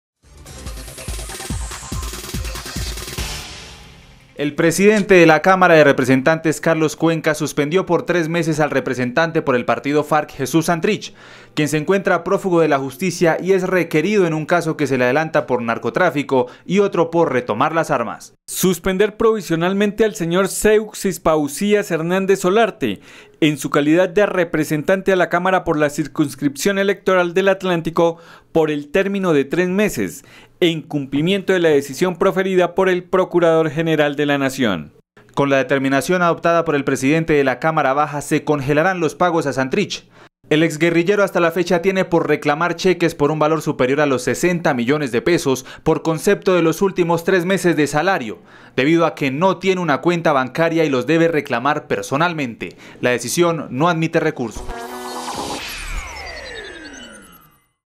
Cámara de Representantes suspende por tres meses a 'Jesús Santrich'
El presidente de la Cámara de Representantes, Carlos Cuenca, suspendió por tres meses al representante por el partido Farc, "Jesús Santrich", quien se encuentra prófugo de la justicia y es requerido en un caso que se le adelanta por narcotráfico y otro por retomar las armas.